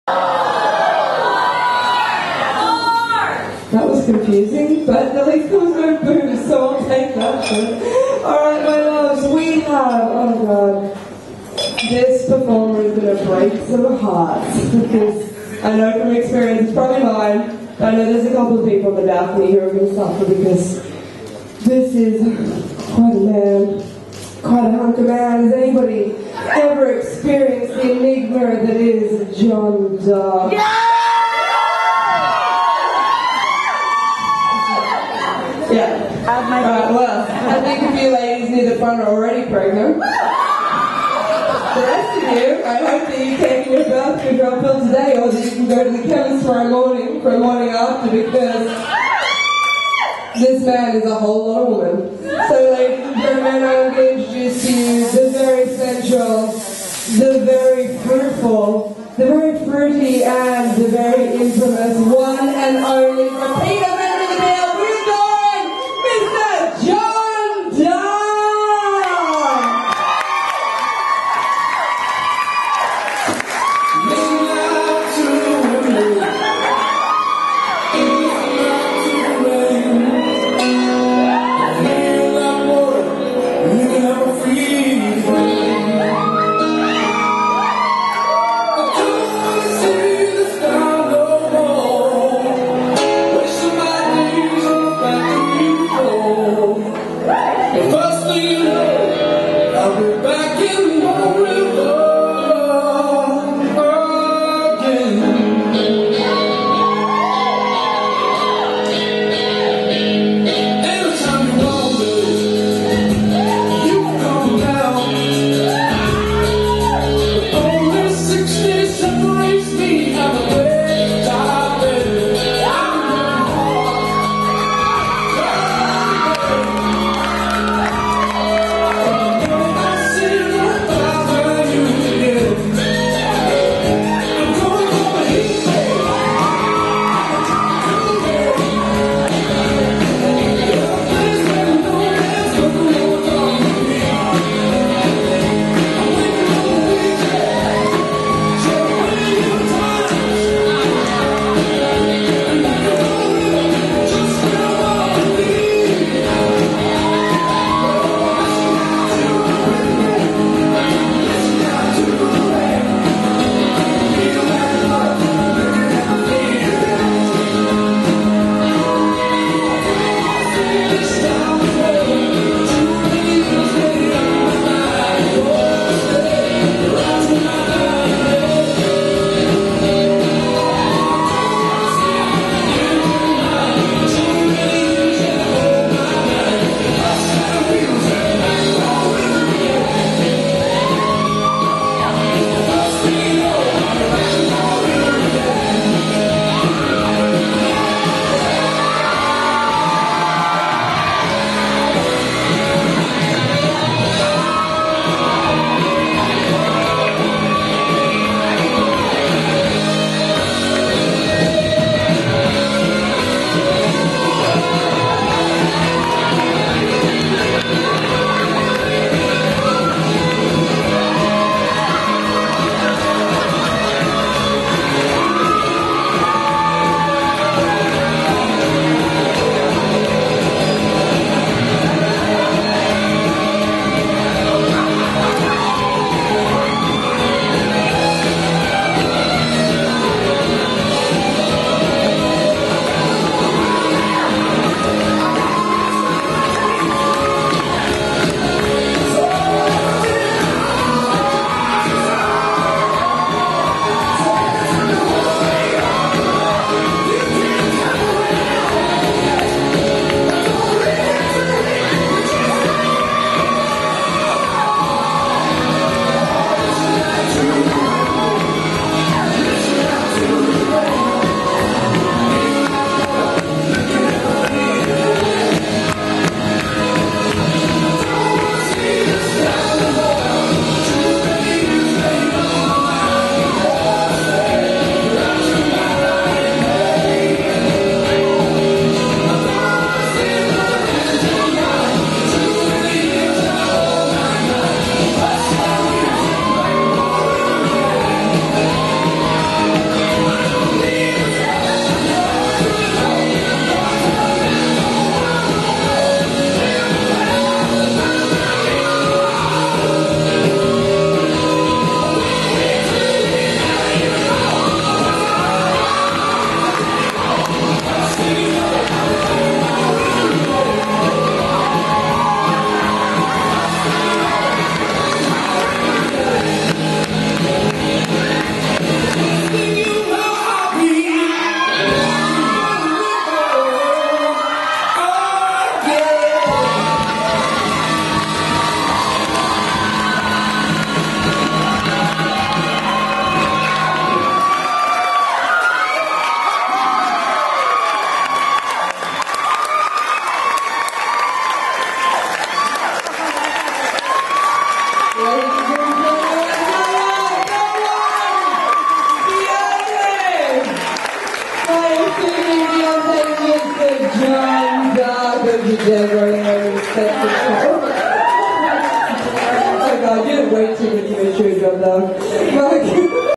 Drag King John Dark aka Lillian Star at The Peel burlesque night Sydney
dirty queer drag king genda benda lillian star amazing show at The Peel burlesque night in Sydney Australia.